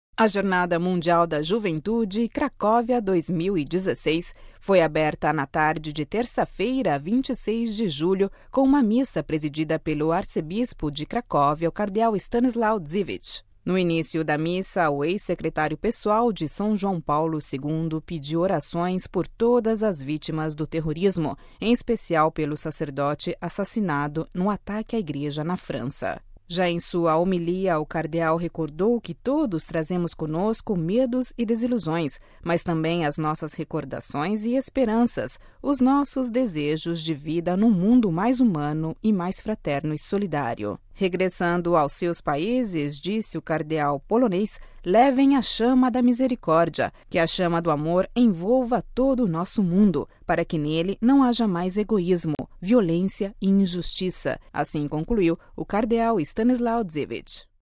Missa de abertura da JMJ 2016
O Arcebispo de Cracóvia presidiu à Missa de abertura da Jornada Mundial da Juventude